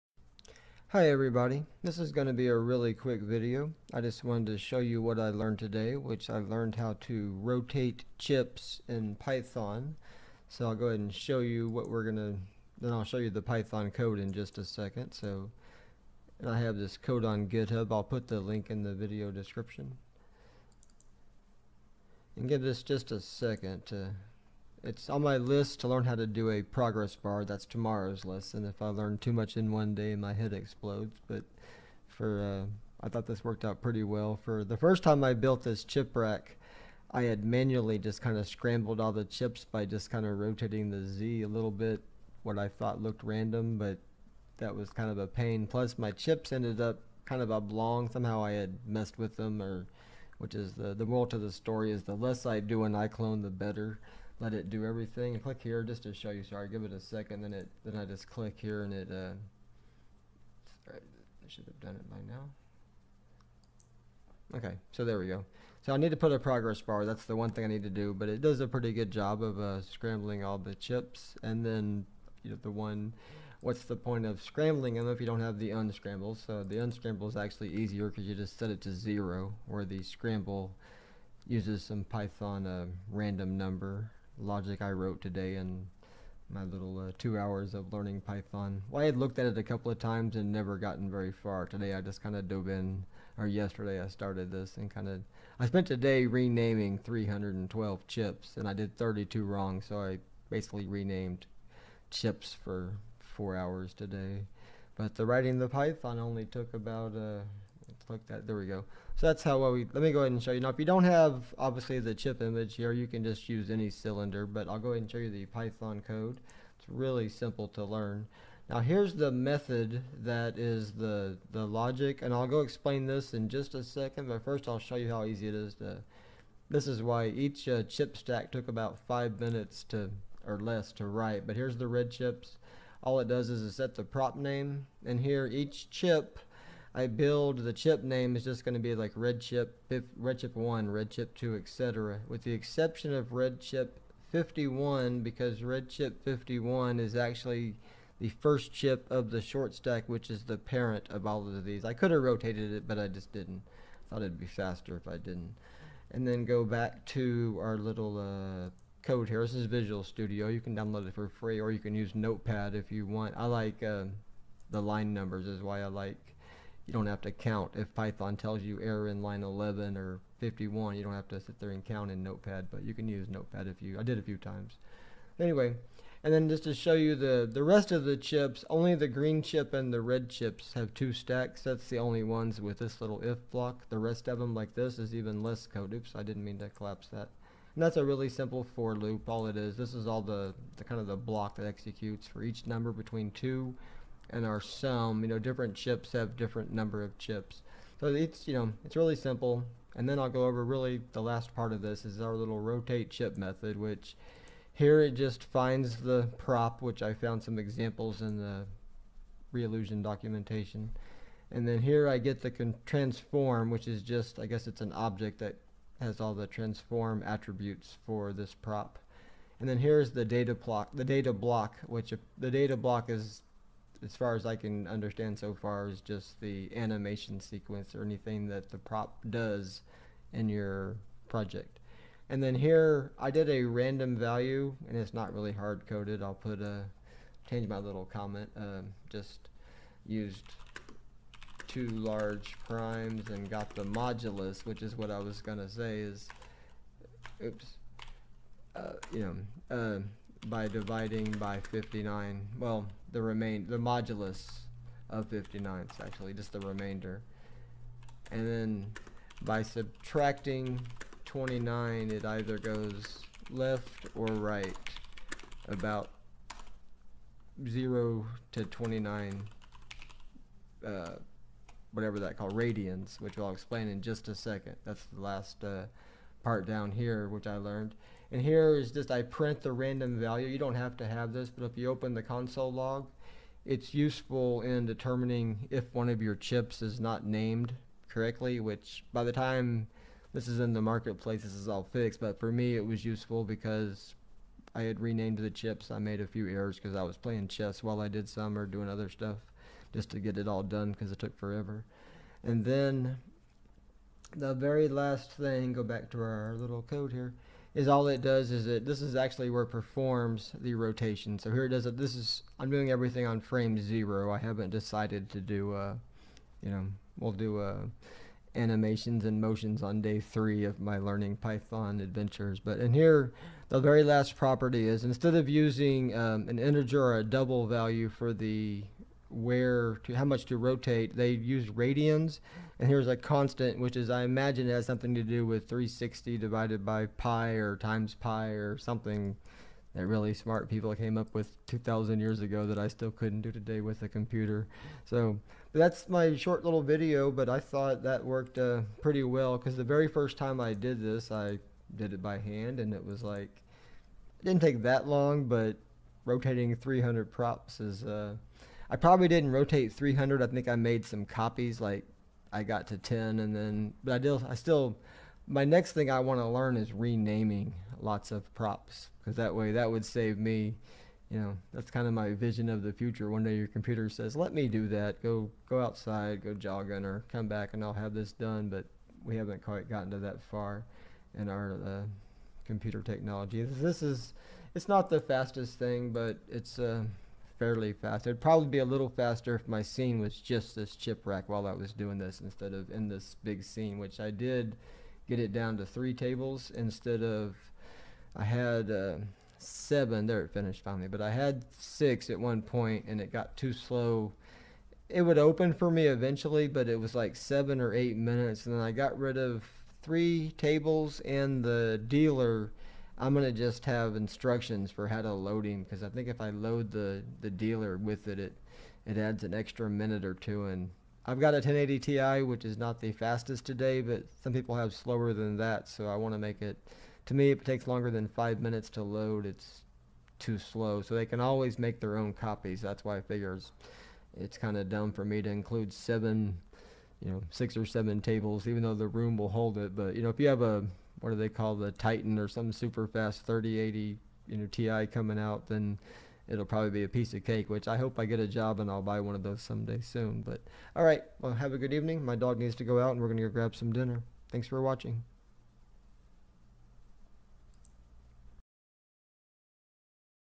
Rotating Props In IClone using Python API
I learned P:ython in a few hours, so I am sure that makes me qualified to teach it.

Here are the links to the scripts shown in the video in case it helps anyone:

My Poker Room will be in the market place in the next day or two, being able to scramble and unscramble the chips was one of the last few things I needed.

I will add a progress bar next, I just haven't had time. Here is the documentation for Python Progress Bars:

Tomorrow I hope to make a new video operating the shuffle machine.

Look for the Poker Room in the market place before New Year's eve.